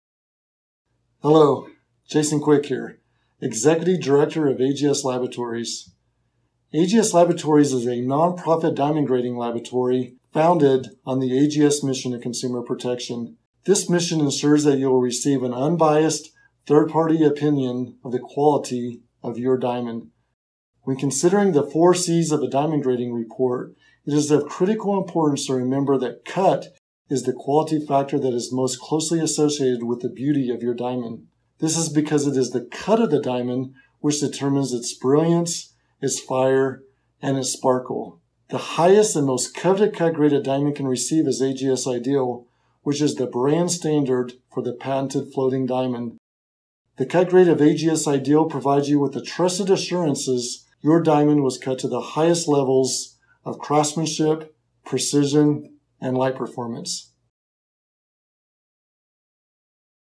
A word from American Gem Society Laboratories | The Floeting® Diamond
Our revolutionary diamond cut saw American Gem Society Laboratories create a new grading standard, specific to the cut. The Floeting® Diamond has achieved the highest and most coveted cut grade a diamond can receive: Triple Zero Ideal, now GIA's Excellent Symmetry.

Formerly Executive Director from American Gem Society Laboratories and now Director of Diamond Cut Research from GIA, Jason Quick, gives an explanation of what it takes to achieve such a high cut grading standard, and their highest cut grade possible.